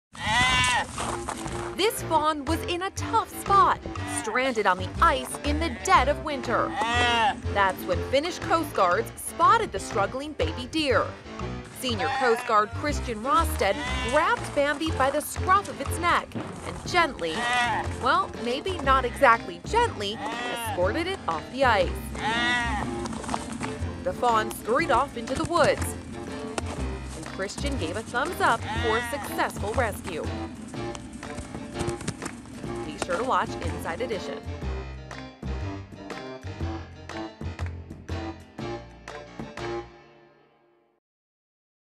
Adorable Baby Deer and Coast Guard Rescuer Scramble to Get Off Ice
A fawn was in a tough spot, stranded on the ice in the dead of winter. That's when Finnish Coast Guard officials spotted the struggling baby deer near the Inkoo Archipelago in southern Finland. Senior Coast Guard Kristian Rastedt grabbed the animal by the scruff of its neck and escorted it off the ice. The fawn scurried off into the woods. And Kristian gave a thumbs-up for a successful rescue.